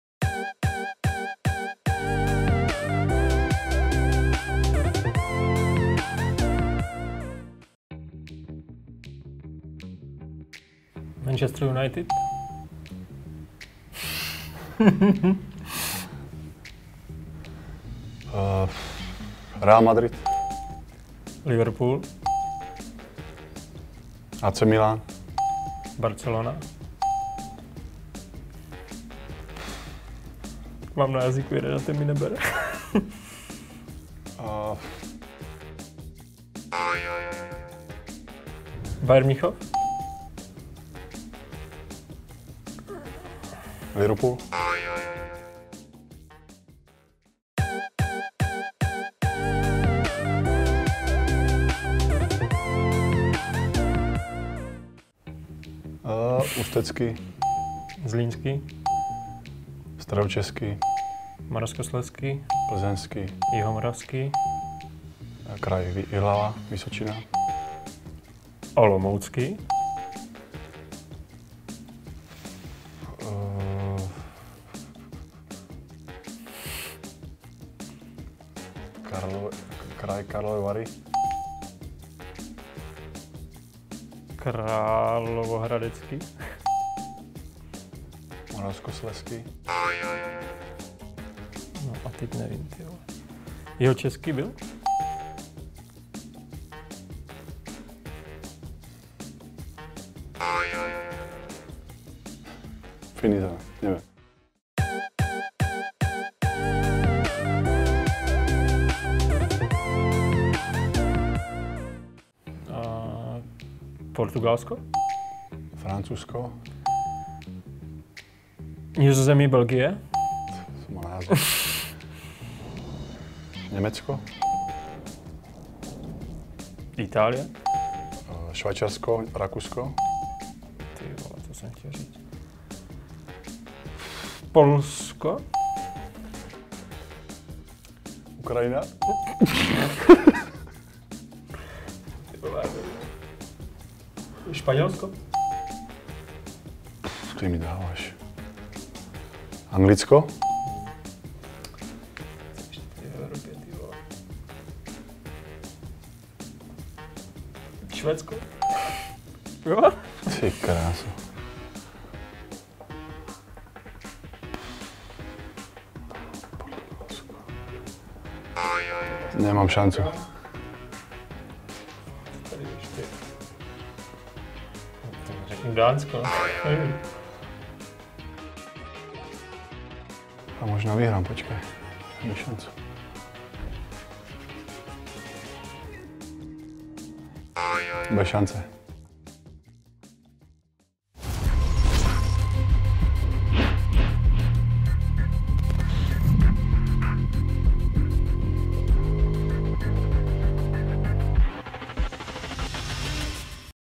Vyjmenovávání ve Zlíně: Lukáš Železník a Matej Rakovan
Další díl Vyjmenovávání tentokrát potrápil hráče FC Fastav Zlín, brankáře Mateje Rakovana a útočníka Lukáše Železníka. Jak se s výzvou popasovali oni?